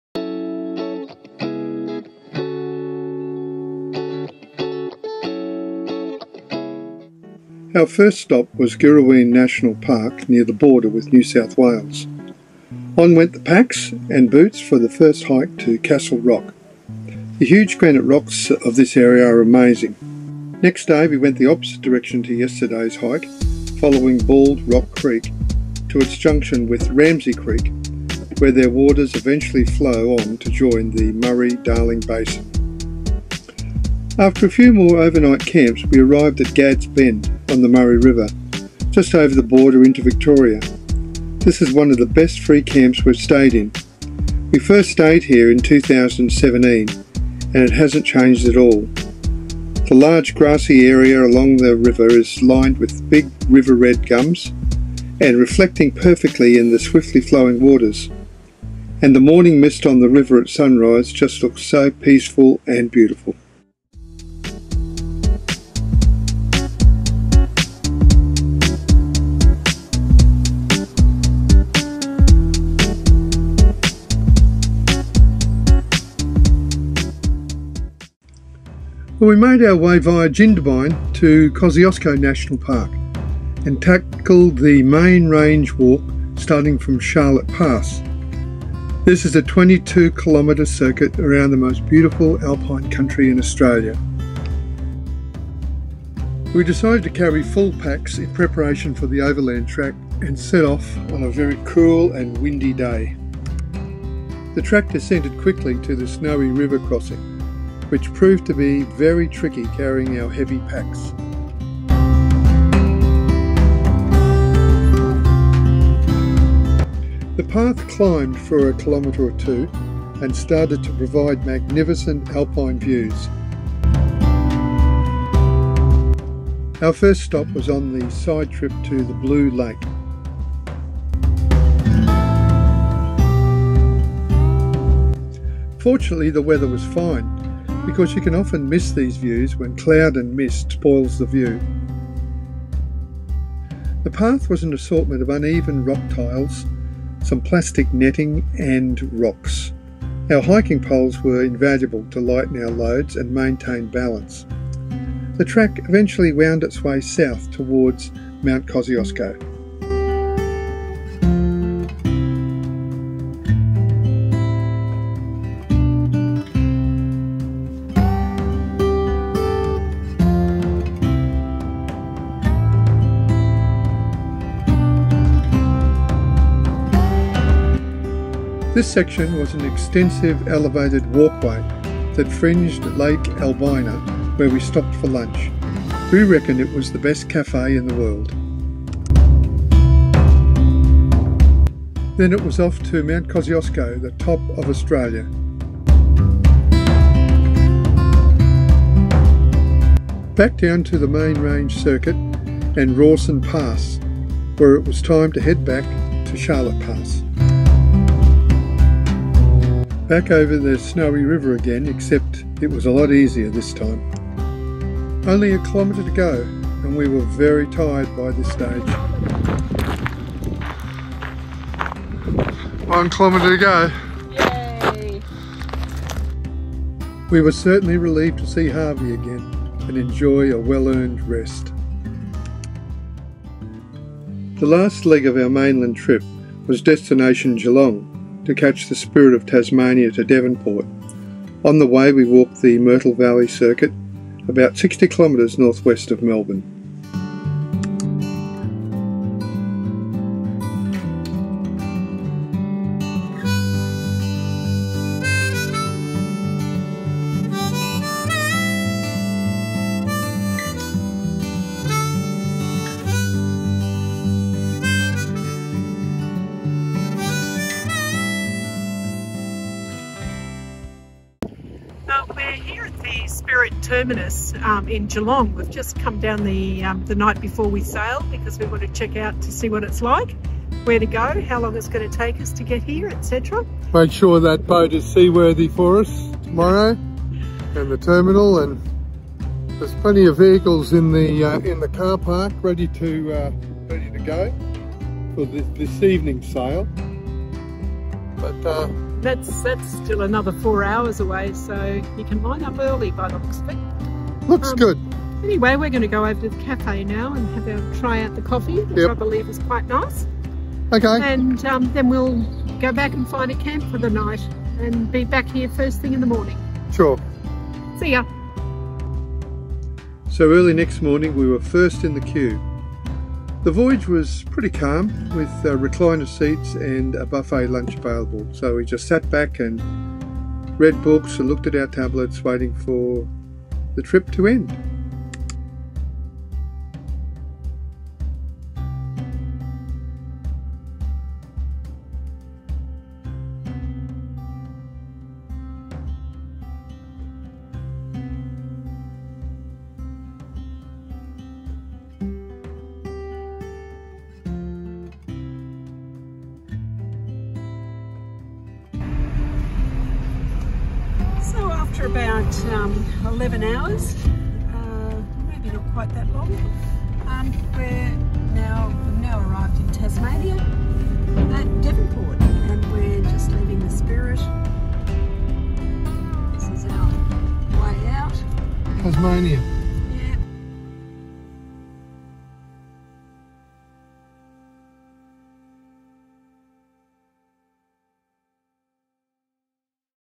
2024-01 SOUTHERN EXPLORER PART 1
This is the first part of our trip south in Harvey, our Explorer motorhome, starting January 2024